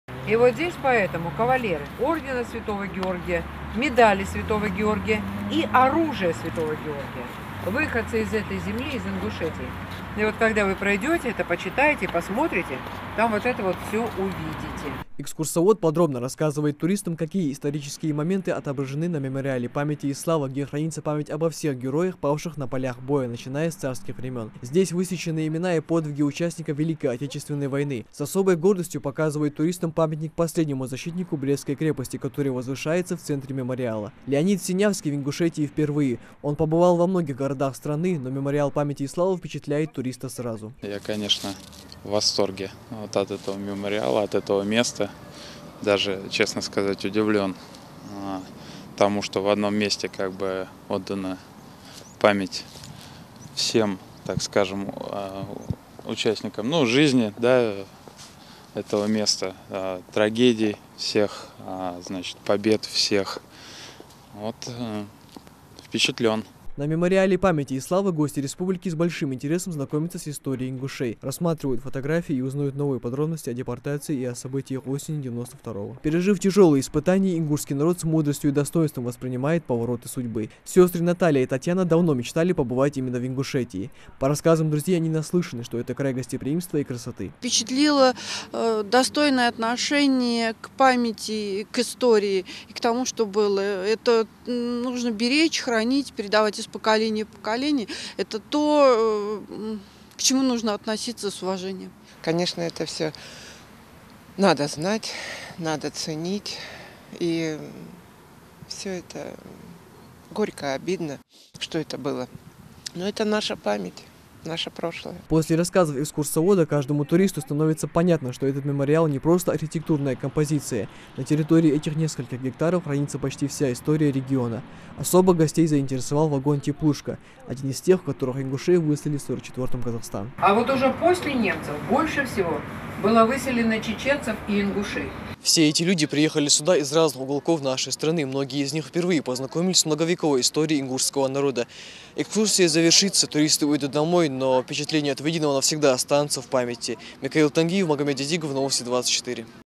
Мемориал памяти славы - одна из главных достопримечательностей Ингушетии.
Мемориал памяти и славы - одна из главных достопримечательностей региона. Расположившись прямо по трассе "Кавказ" он привлекает к себе внимание не только туристов, но и просто проезжающих мимо людей. Еженедельно,памятник посещают десятки гостей из других регионов.